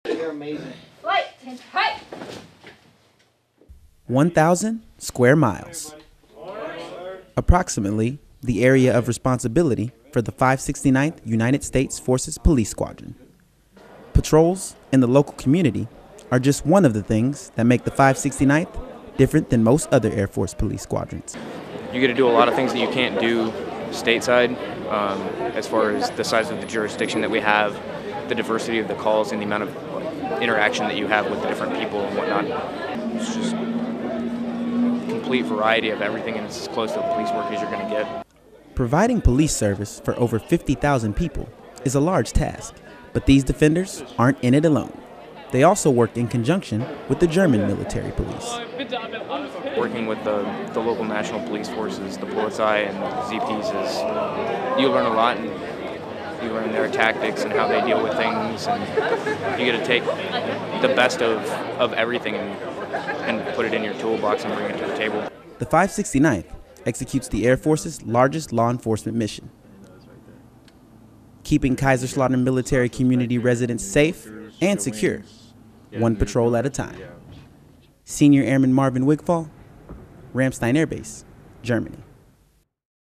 Security Forces Ride Along
The Kaiserslautern Military Community is the largest American community outside the United States. Senior Airman Marvin Wigfall talks about a squadron that dedicates its time to keeping the over 50,000 thousand Americans that live in the KMC safe.